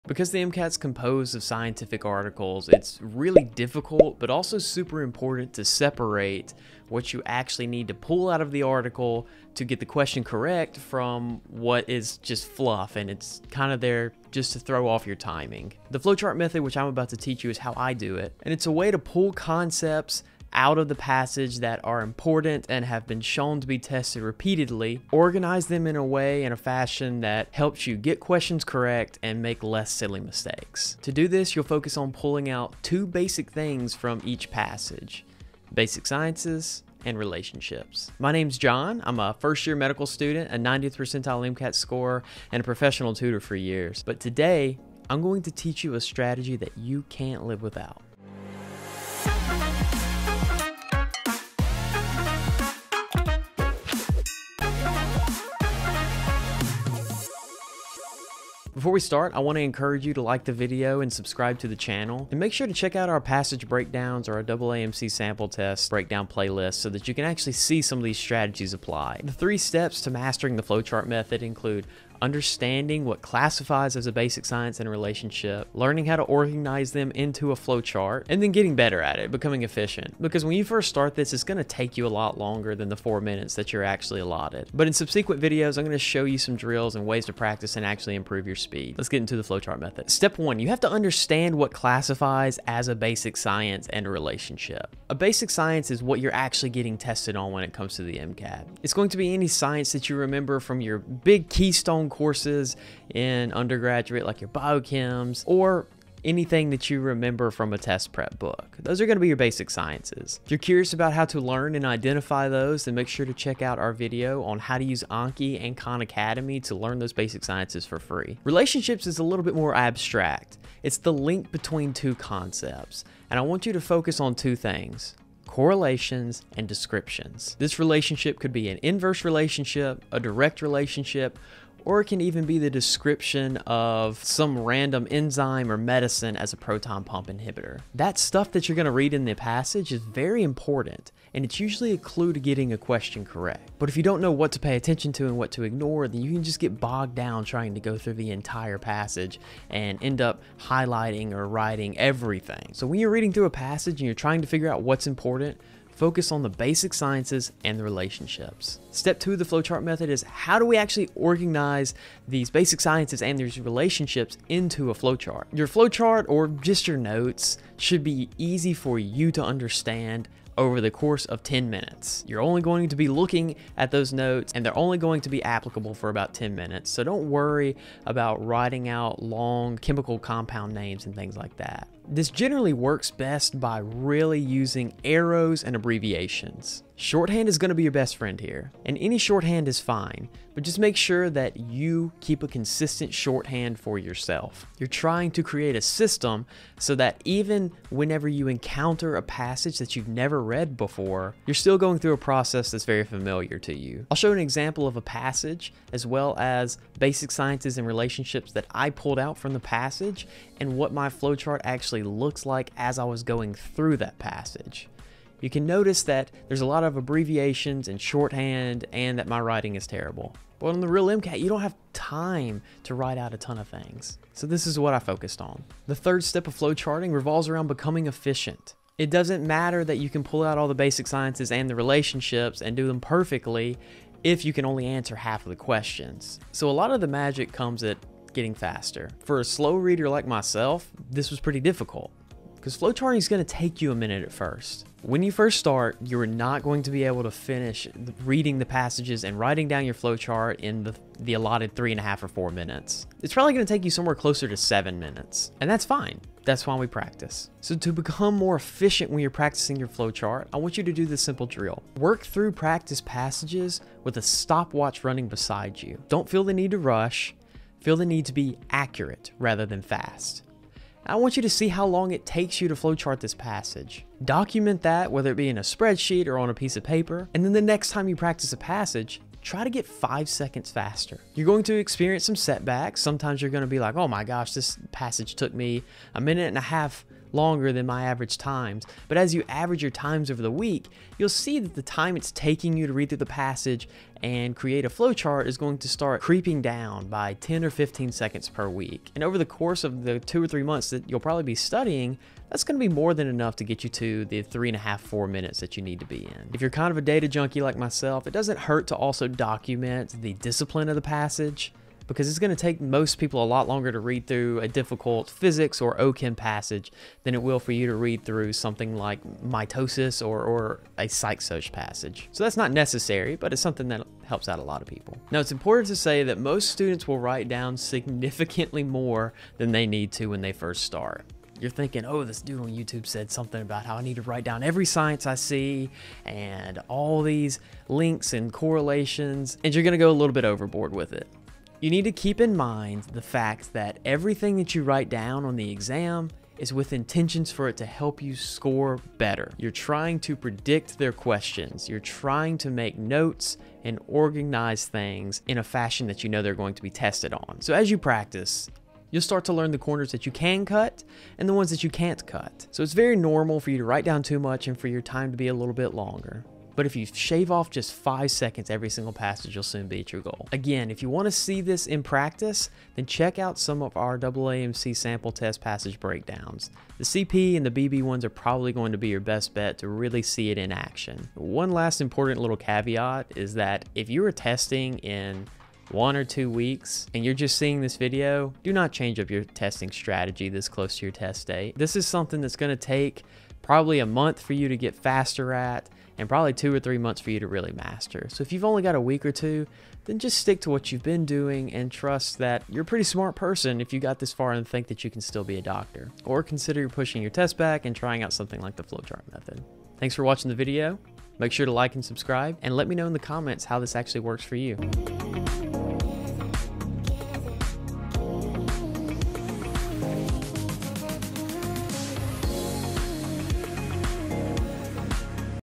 Make MCAT Passages EASY - MCAT Strategy - Flowchart Method
--- Lectures, books, questions & more! Check it out at link above 👀^^^

The best kept secret among high-scoring MCAT testers and tutors is how we go through the passage. Some people call this "passage diagramming" or "passage mapping", but we call it the Flowchart Method. Join John as he teaches you his favorite strategy and shows you how to boil down any MCAT passage into its simplest and most useful components.

Timestamps:
0:00 Intro
1:09 Flowchart Method Overview
1:30 Step 1 - What Do I Write Down?
2:35 Step 2 - Organizing Into Flowchart
3:25 Example of Flowchart Method
3:47 Step 3 - Getting faster WITH DRILL
6:20 When NOT To Use This Strategy

We'll keep posting videos as the demand of medical school allows us :)